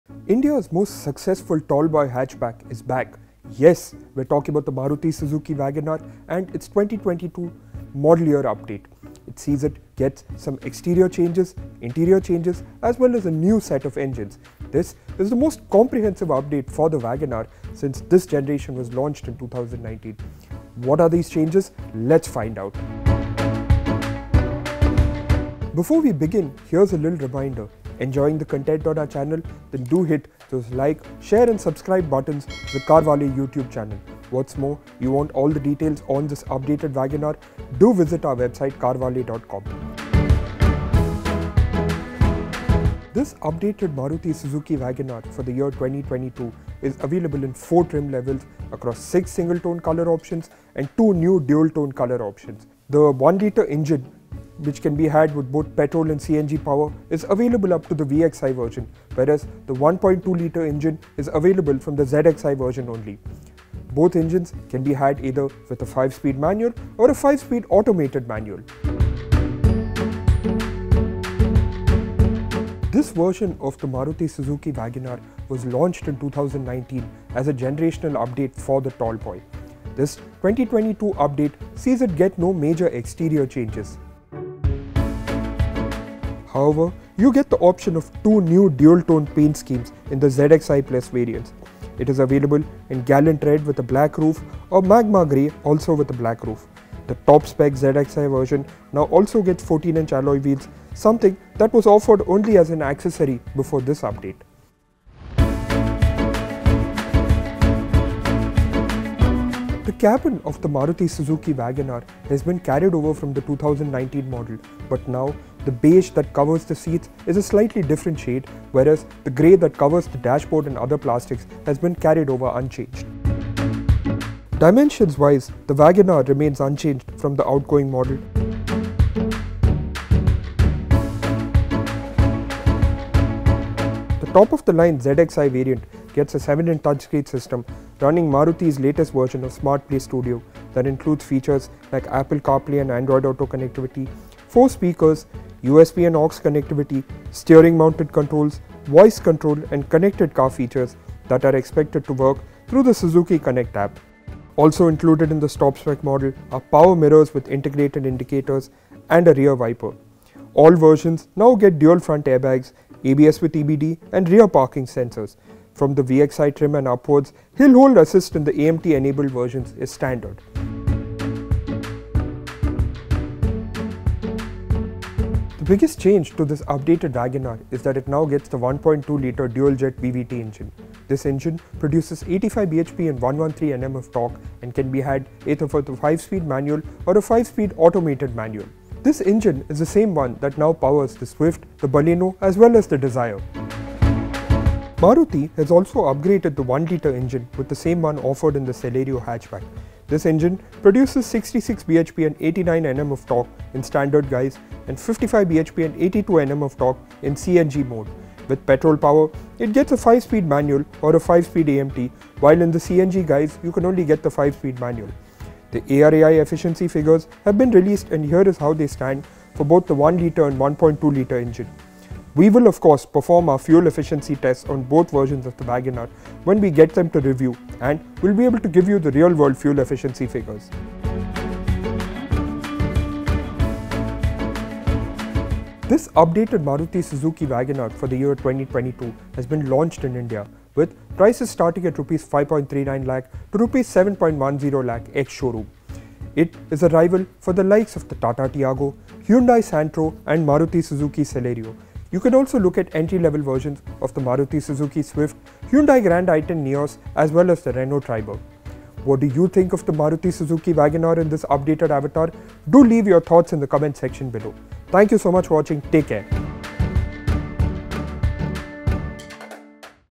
Maruti Suzuki Wagon R 2022 Model Launched | Price, Features, Mileage and Details Explained | CarWale
Maruti has launched the 2022 model year version of the popular Wagon R tall-boy hatchback. Venkat finds out what's new and if you should take notice.

00:00 Introduction
00:25 Like, Comment, Share, Subscribe and Click the Bell Icon!
00:34 Maruti Suzuki Wagon R 2022 Details at CarWale.com
00:40 Variant Details - Introduction
00:47 Colour Options - Single Tone
00:51 Colour Options - Dual Tone
00:53 Drivetrain Options
01:09 Exterior Changes - Introduction
01:25 New Dual Tone Colour Options
01:37 New 14-inch Alloy Wheels
01:45 Interior Changes - Introduction
01:54 New Interior Theme
01:59 Dashboard
02:03 Dimensions vs Santro vs Tiago vs Celerio
02:14 Smartplay Touchscreen Infotainment System
02:27 Steering Mounted Controls
02:30 Suzuki Connect
02:34 ORVM with Integrated Indicators
02:39 Rear Windshield Wipers
02:40 Dual Front Airbags
02:43 Rear Parking Sensors
02:45 Hill Hold (for AMT only)
02:51 New Engine and Transmission - Introduction
02:55 New 1.2-litre DualJet Engine
03:14 New 1.0-litre DualJet Engine
03:36 Mileage - ARAI Certified
03:52 Price and Competition - Introduction
04:00 Ex-Showroom Price
04:06 Competition
04:19 Comment Below!
04:25 Up Next